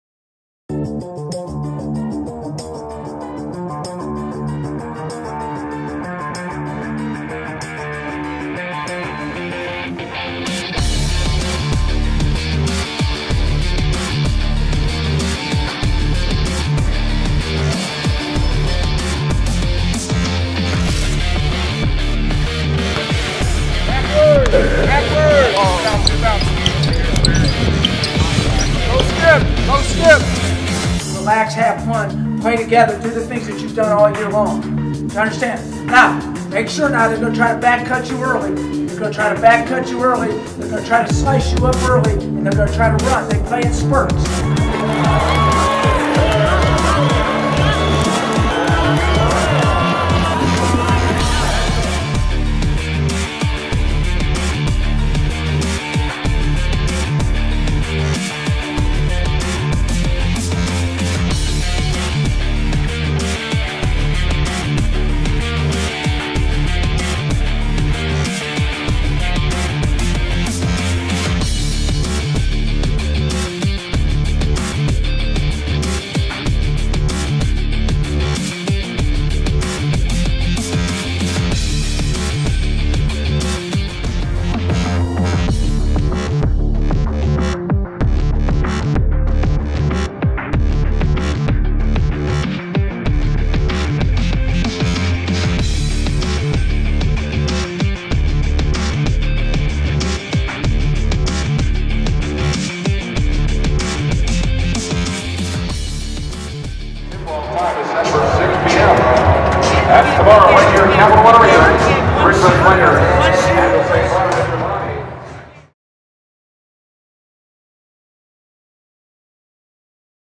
Behind the Scenes - @DuqMBB Takes on Richmond in A-10 Tournament
The Duquesne men's basketball team fell to Richmond in the 2018 Atlantic 10 Tournament on Thursday evening at Capital One Arena in Washington, DC.